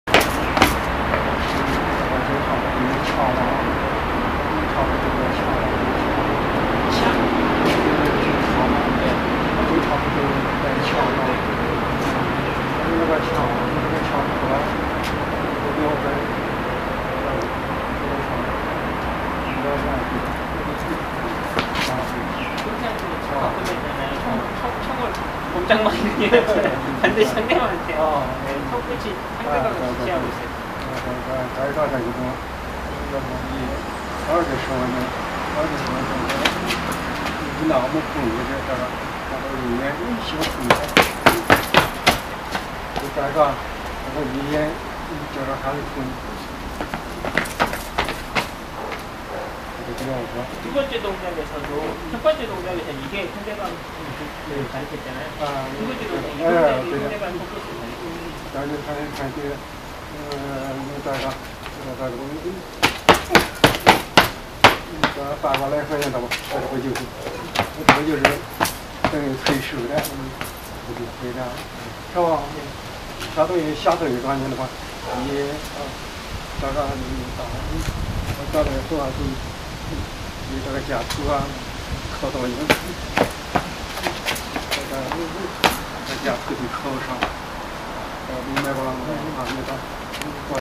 Chen shi taijiquan fajin 금강도대 발경
Jung Oneil master
 Yuiltaijicenter   Chen Style Taijiquan Practical Method
His teacher is Fan DeChen
Fan laoshi learned from Hong JunSheng for 30 years
2017, 11 in Shangdongsheng